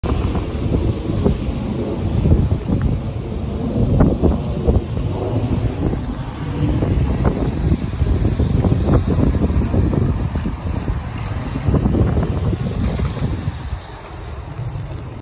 MTA NYCT 1/16/2024 | 2018 Novabus LFS On The Bx18A Via University Ave
Time was, 6:24 PM